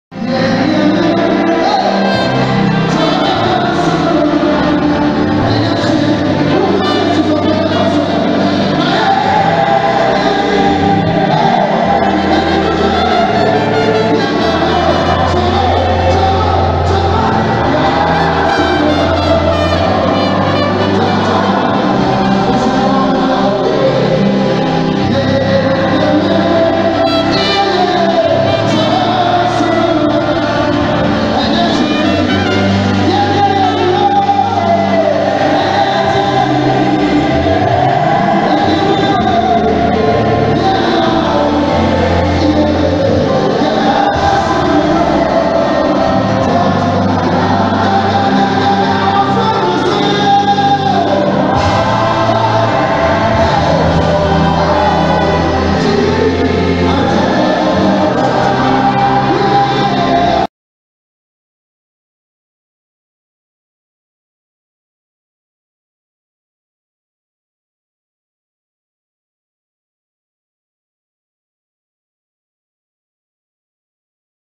SK FRIMPONG LIVE AT THE BREAKTHROUGH MINISTRIES
A LIVE WORSHIP AT THE BREAKTHROUGH FAMILY MINISTRIES WITH SK FRIMPONG ...THE DAVID OF OUR TIME